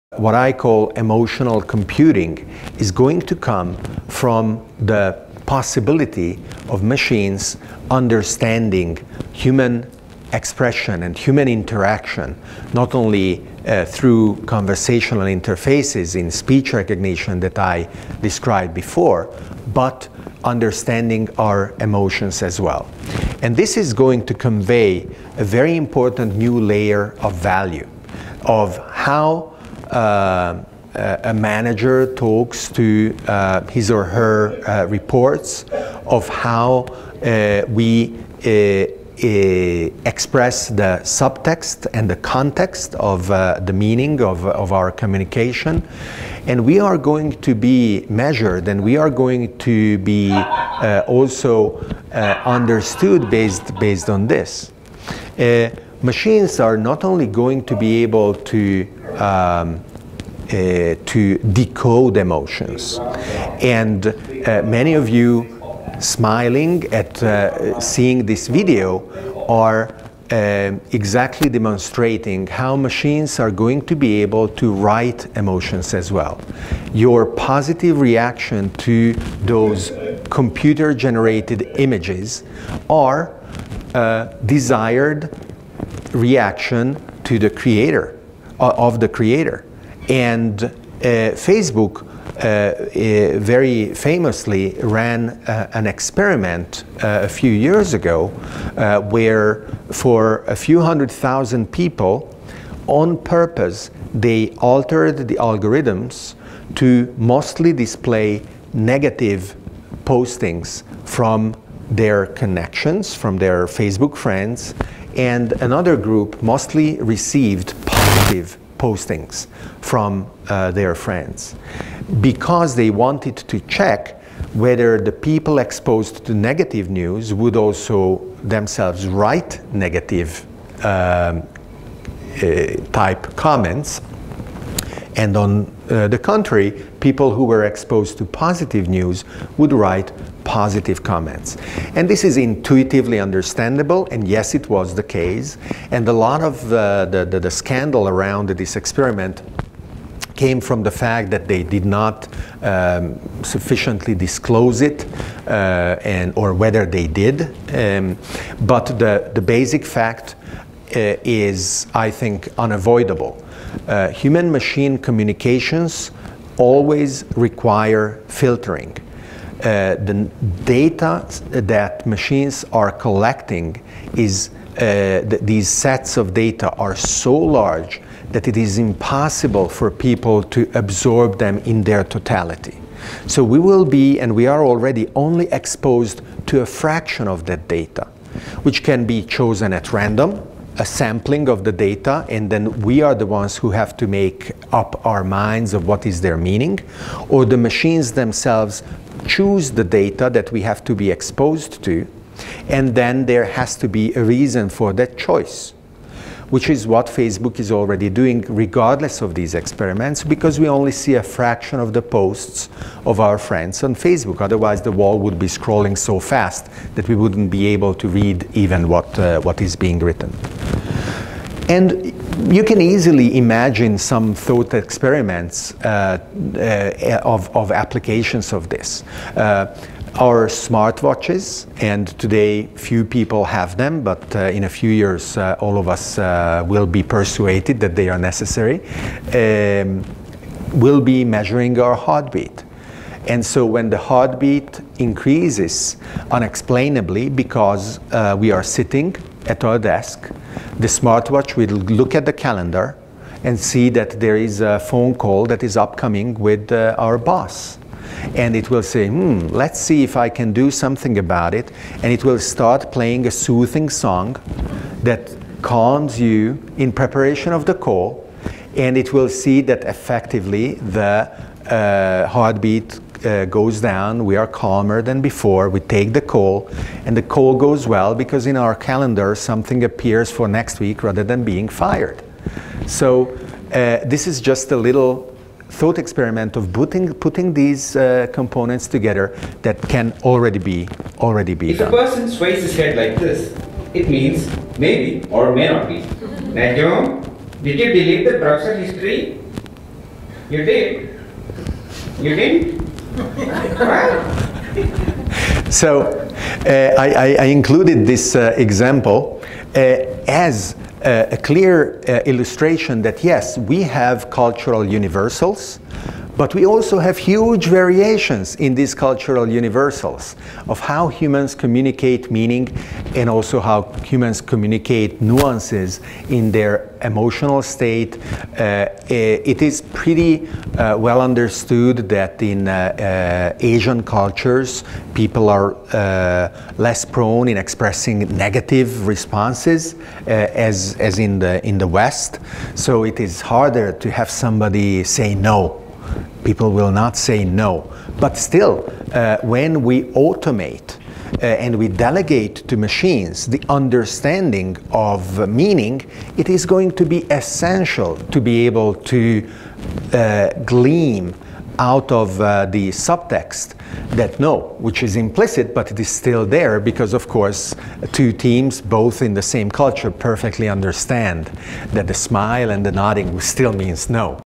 Emotional computing
Clip from the opening talk at TC World India on March 13, 2015. "Smarter Global Systems: Emotional Computing And the Challenge of Deeply Integrating Computers In Our Decisions

Next generation IT systems will be smarter that todays, packing the power of supercomputers in dust-sized packages. Their ability to understand their environments, and people is going to be a necessary part of their value. Emotions, intentions, goal-seeking, are all human expressions and activities that will become accessible to computers. How can the localization industry leverage this change? What are the global cultural invariants that get preserved, what are the peculiar ways the societies organize themselves and must be reflected in the differences in these systems’ behaviors? Which types or approaches will generate the most resilient environments in which people will thrive?"